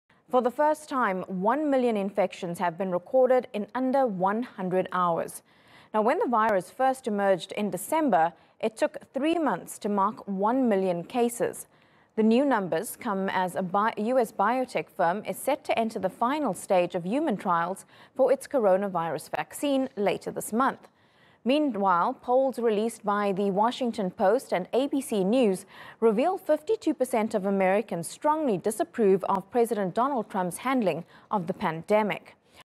One million Covid-19 cases recorded in under 100 hours
Coronavirus Pandemic